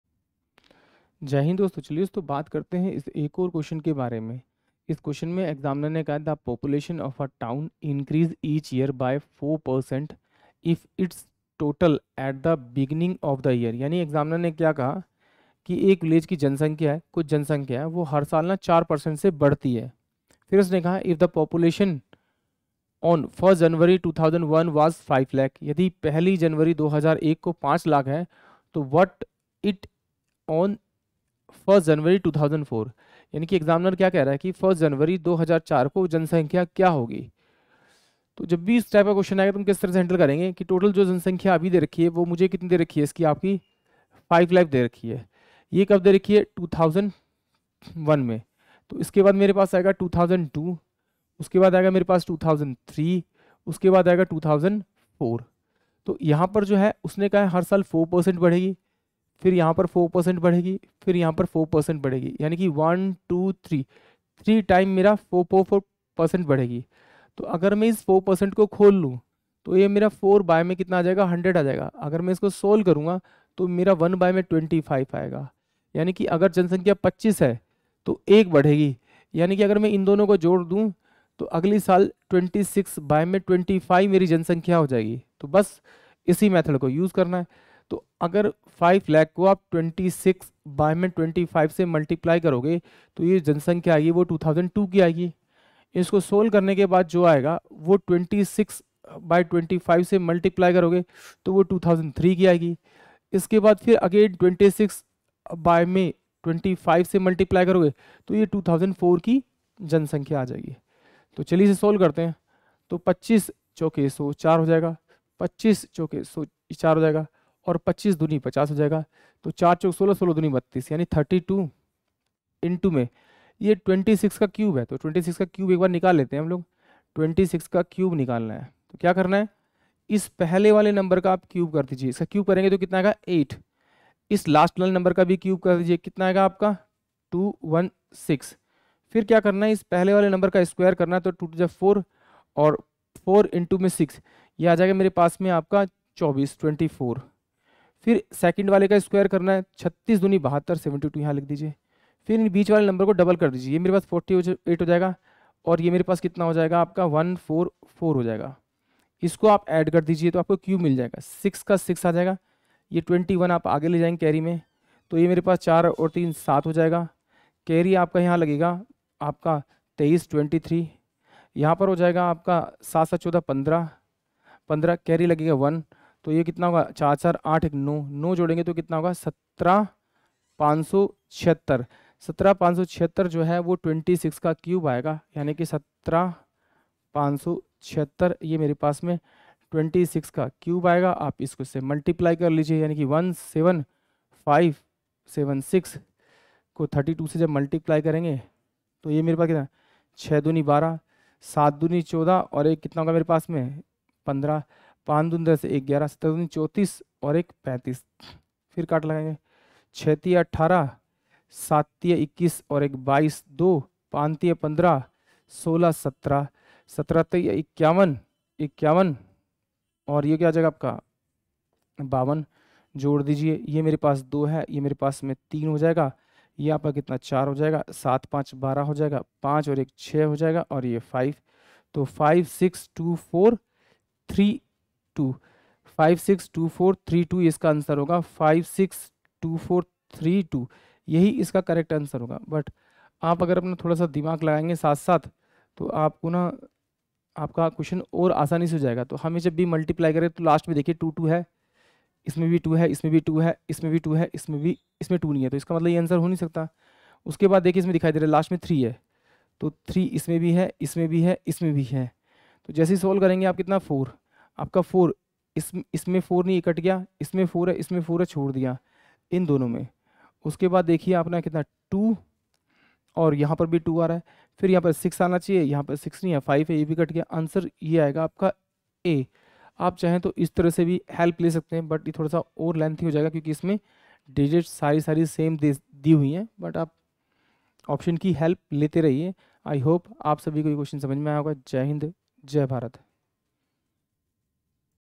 The population of a town Increases each year by 4% of its total at the beginning#percentage
The population of a town Increases each year by 4% of its total at the beginning of the year. If the population on 1 January 2001 was 500000, what was it on 1st January, 2004?एक कस्बे की जनसंख्या प्रत्येक वर्ष वर्ष की शुरुआत में उसकी कुल जनसंख्या की 4% बढ़ जाती है। यदि 1 जनवरी 2001 को जनसंख्या 500000 थी, तो 1 जनवरी 2004 को यह क्या थी?
उ. प्र. पुलिस